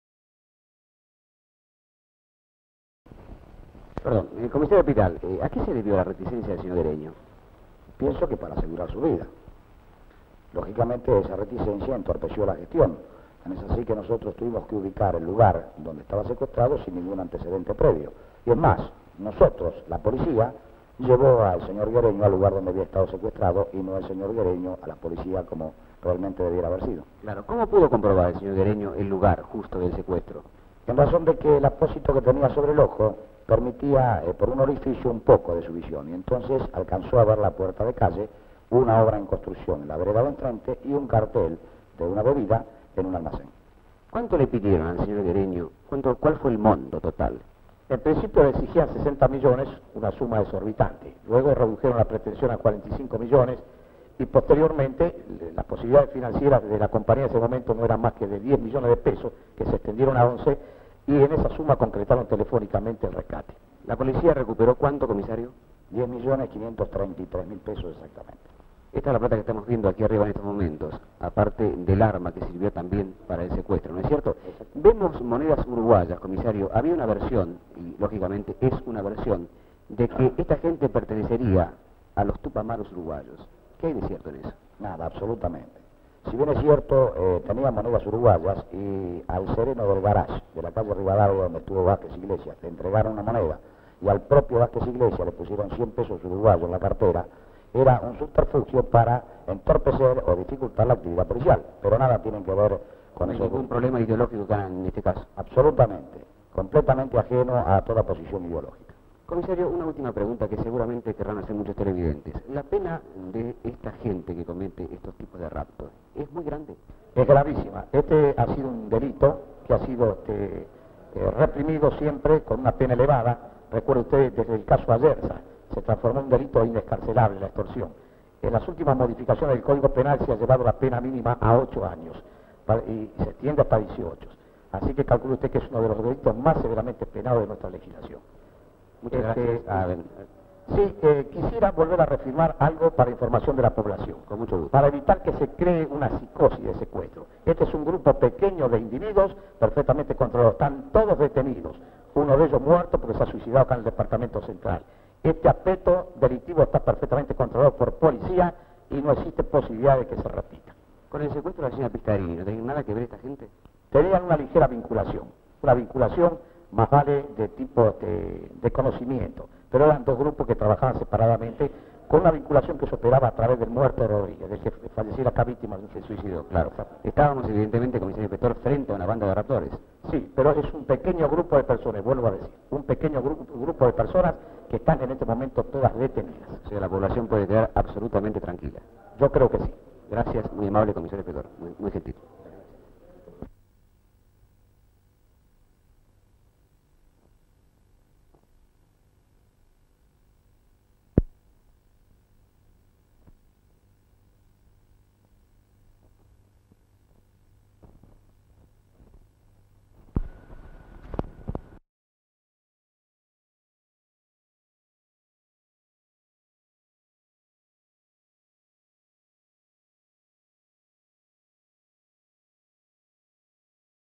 DiFilm - Nota a comisario por secuestro de Jose Abel Guereño 1969
Bs. As.: Nota periodística en el Departamento Central de la Policia Federal Argentina. Reportaje al comisario Piran, en el cual se le pregunta: -¿A que se debió la requisa del señor Jose Abel Guereño? -¿Cómo pudo comprobar el señor Guereño el lugar justo del secuestro? -¿Cuánto dinero le pidieron al Sr. Guereño, cual fue el monto total? -¿La policía recupero cuanto dinero? -¿Esta es la plata que estamos viendo aquí arriba en este momento a parte del arma que sirvió también para el secuestro? -¿Vemos monedas uruguayas, habría una versión, lógicamente es una versión de que esta gente pertenecería a Tupamaros uruguayos, que hay de cierto en ello? -¿No hay ningún problema ideológico en este caso? -¿La pena de esta gente que comete estos tipos de raptos es muy grande? -¿Con el secuestro de la señora Maria Cecilia Pistarini no ha tenido nada que ver esta gente? Primeros planos de los secuestradores Ricardo Lapagiosti y Edgardo Abel Autec detenidos en la seccional policial. (Tico Rodríguez Paz)
Fecha: 29/8/1969
Duración: 3 minutos 57 segundos
Código del film: B-01752